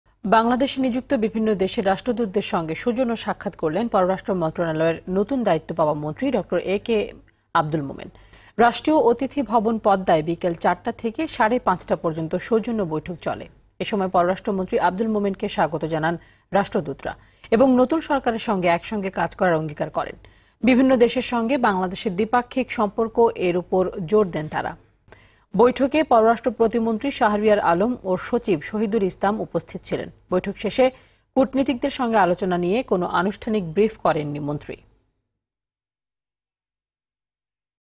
বিভিন্ন দেশের রাষ্ট্রদূতদের সঙ্গে সাক্ষাৎ করলেন পররাষ্ট্র মন্ত্রী ডক্টর এ কে আবদুল মোমেন
News : বিভিন্ন দেশের রাষ্ট্রদূতদের সঙ্গে  সাক্ষাৎ করলেন  পররাষ্ট্র মন্ত্রনালয়ের মন্ত্রী ডক্টর এ কে আবদুল মোমেন

- Team RTV